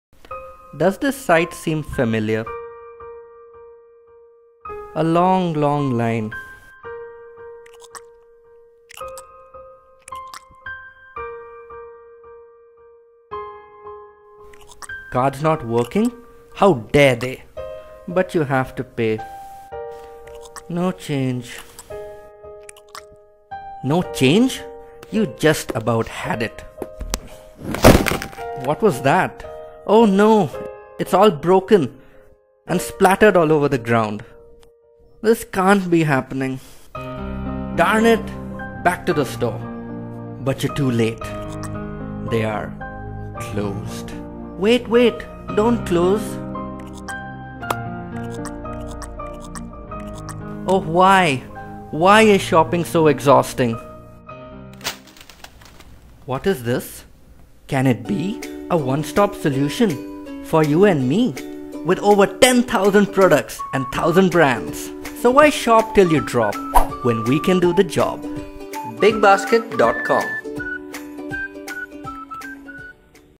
bigbasket com video contest PGA
First attempt at Flash animation for a contest by Prachi K Nagwekar and Girish Chandra T with voiceover by Avijit Chander.
Credits :